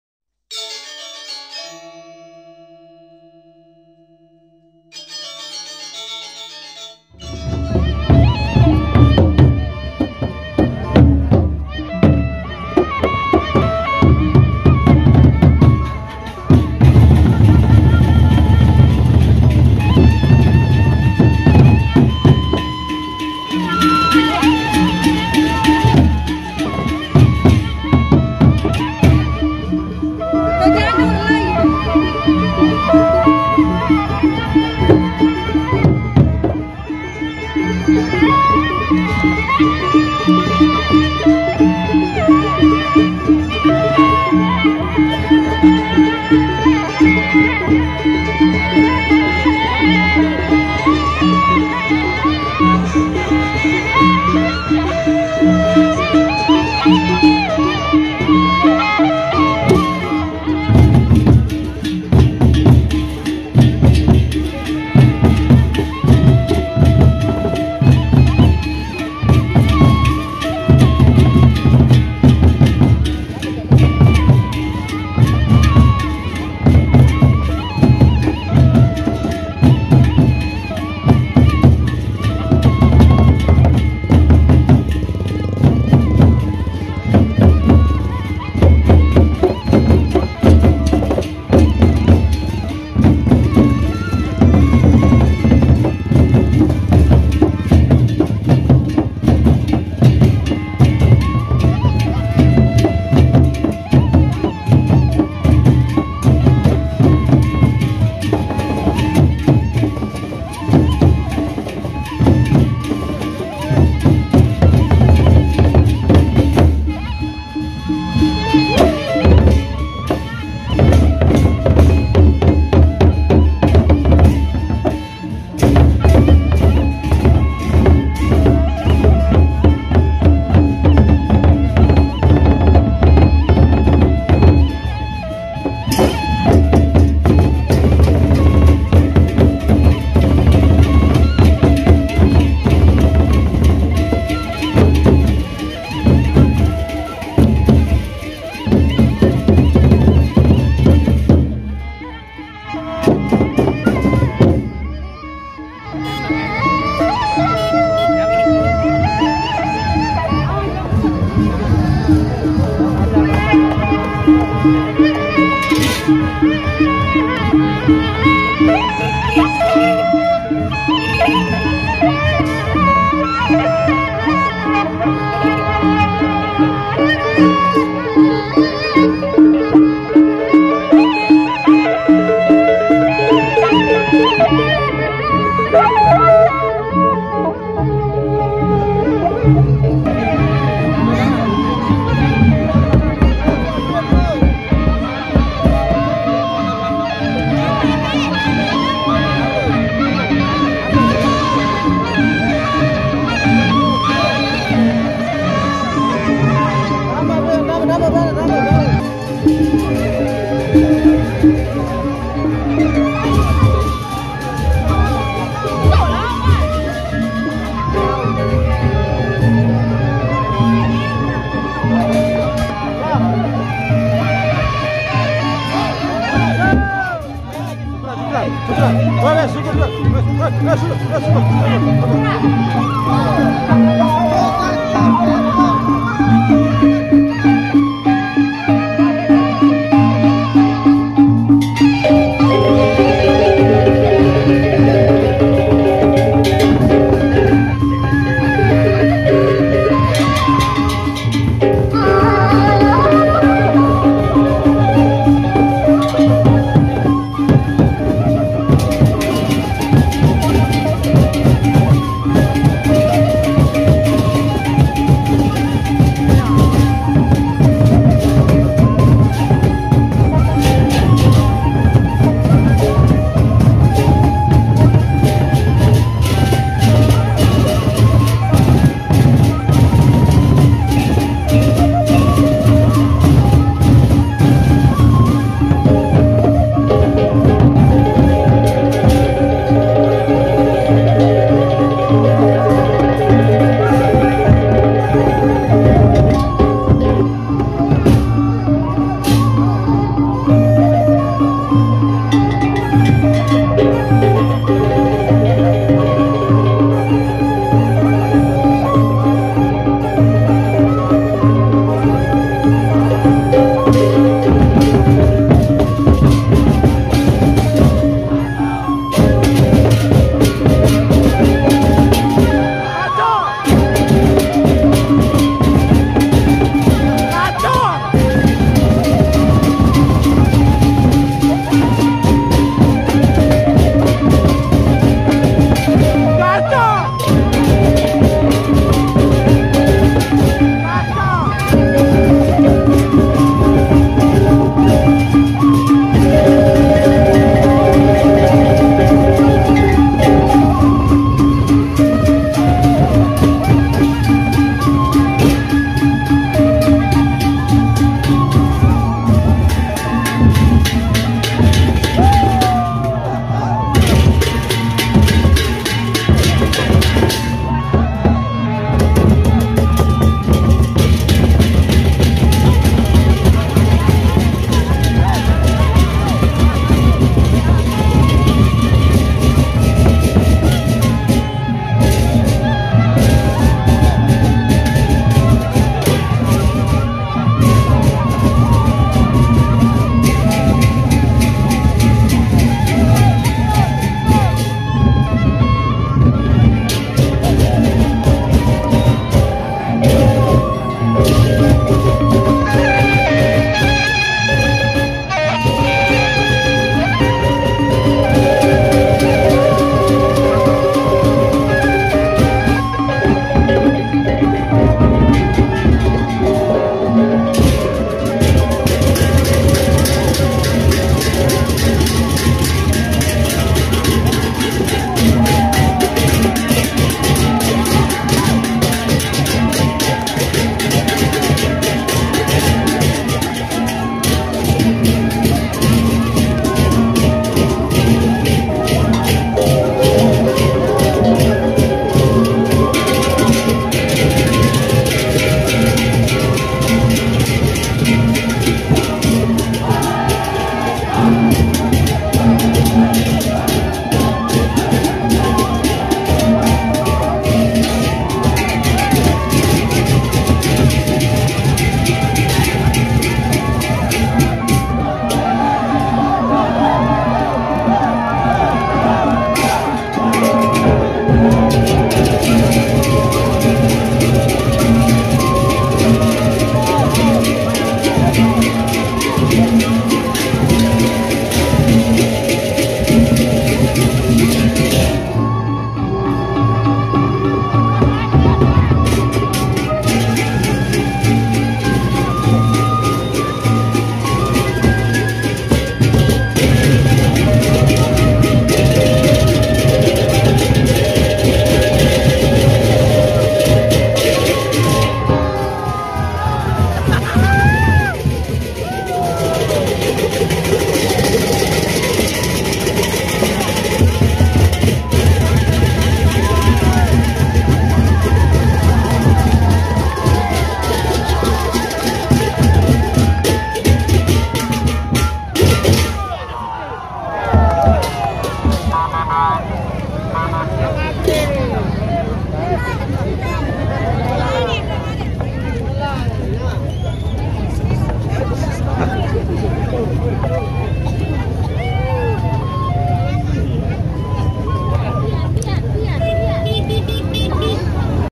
TIGA GENDANG BELEQ ADU VARIASI GACOR LANGGENG BUANA GULUNG-BAJANG EWER-BERIUK GIRANG LEKOR DI londar
TERIMAKSI
UDH NONTON DI VIDIO KAMI..
BAJER OFFICIAL..